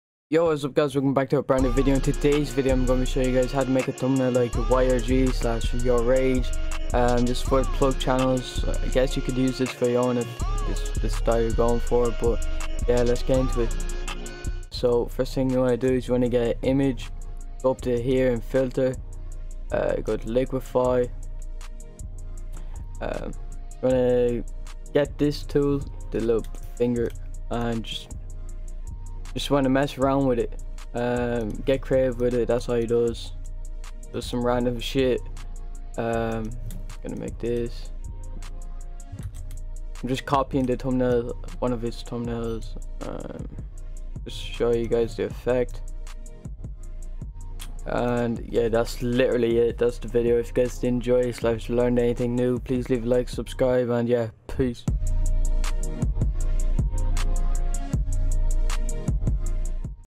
How To Make A Distorted YourRage Gaming Thumbnail in Photoshop 2022! (Plug Channel Thumbnails)
How To Make A YRG AKA YourRage Gaming Thumbnail in Photoshop!

I Hope you guys enjoyed todays photoshop thumbnail tutorial going over How To Make A Distorted YourRage Gaming Thumbnail in Photoshop which is easy to follow for plug channels/beginner designers! I will be making some more cool reaction/ plug channel thumbnail tutorials along with some banner tutorials!